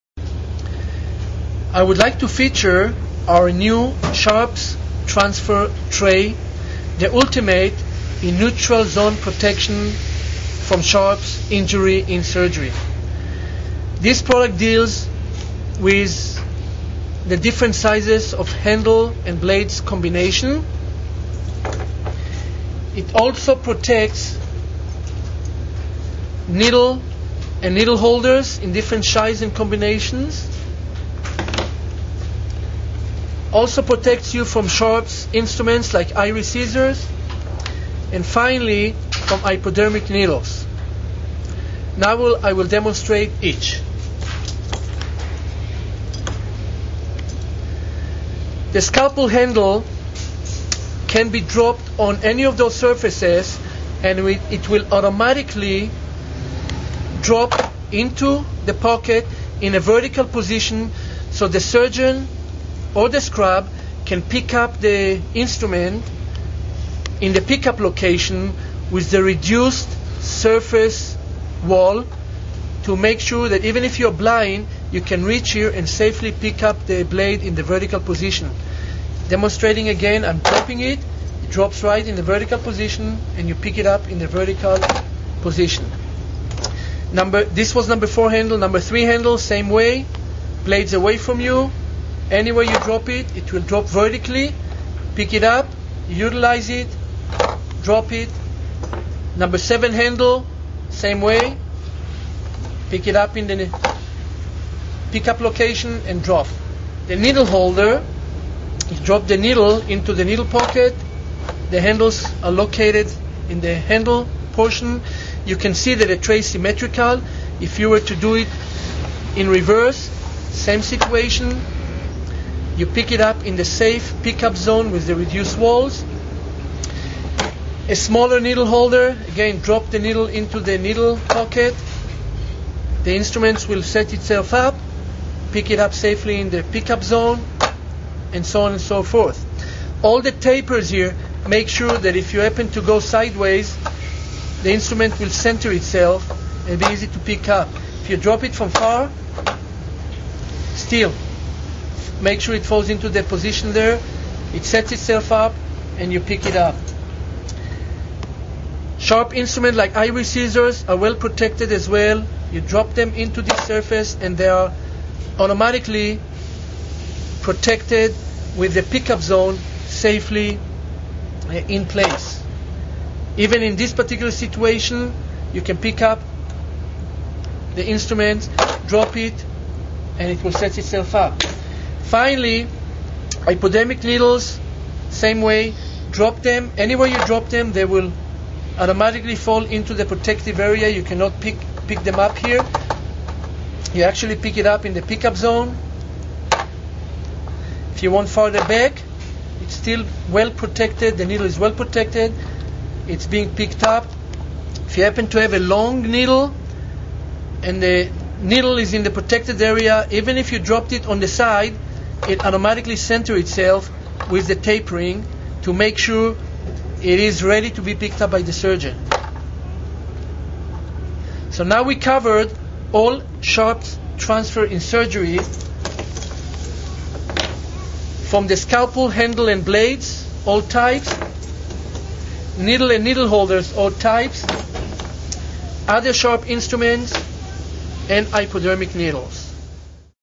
Neutray™ Pat. Pend.
The "Neutral" Zone - Sharps Safety Transfer Tray - The "Purple" Zone.

Disposable tray for use to transfer and pass commonly used sharp instruments such as scalpels, needle / needle holders, sutures, syringes, Trocars, forceps, K-wires and retractors.